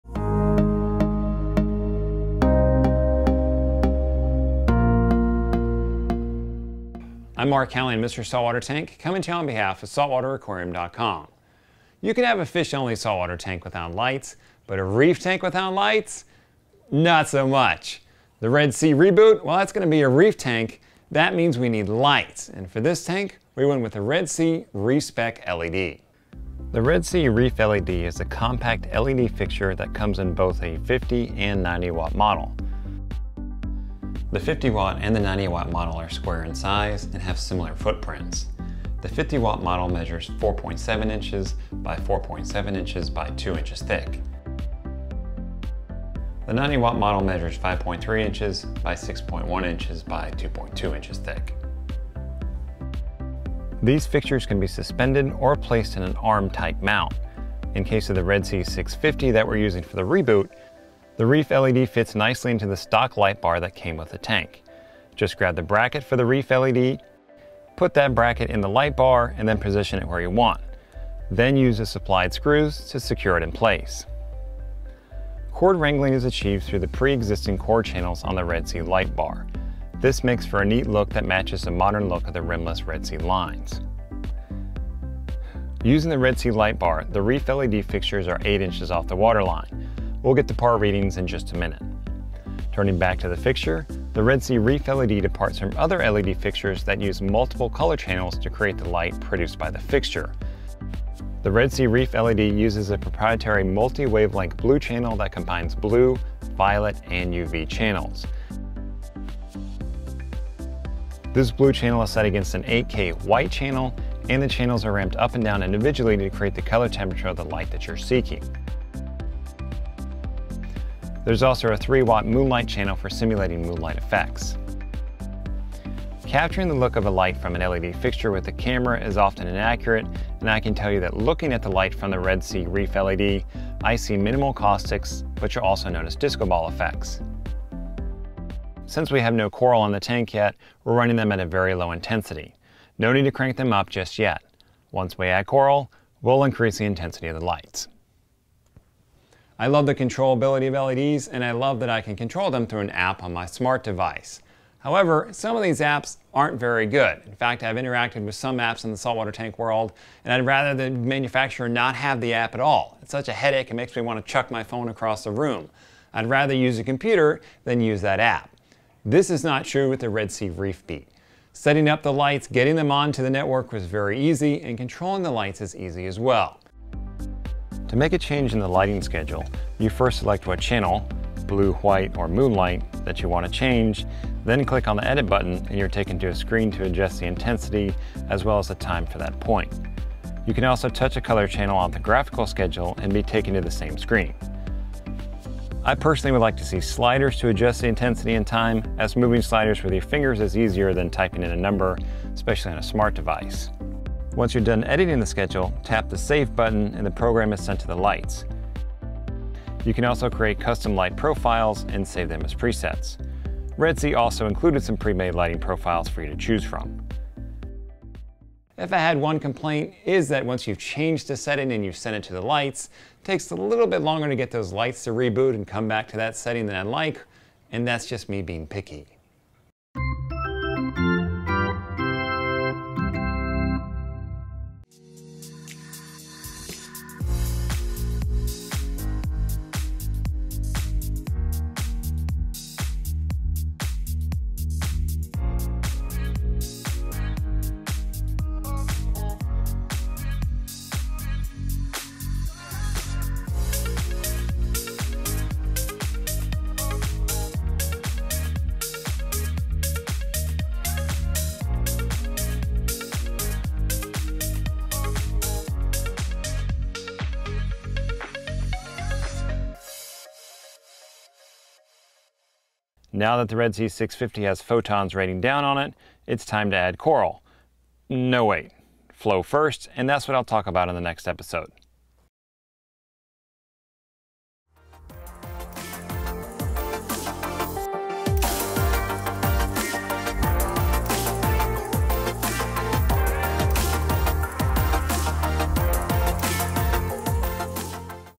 LIGHTS! See What Lighting System We Pick for This Reef Tank! - Red Sea Reboot
In this episode, Mark discusses our lighting choice for the Red Sea Reboot. He'll share which lighting system we chose (Reef Spec LED!)and why, as well as how we're mounting them, their par measurements, and controllability. He'll also give a brief look at how the app works and what he'd change about it.